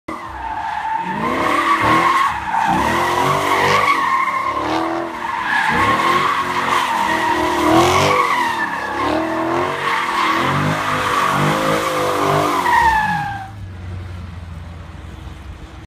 Challenger SRT 392 Scat Pack Shaker- Donuts!
Jeff Glucker from Hooniverse.com gets a Challenger SRT 392 Scat Pack Shaker a bit loose with donuts from a video on the Hooniverse YouTube channel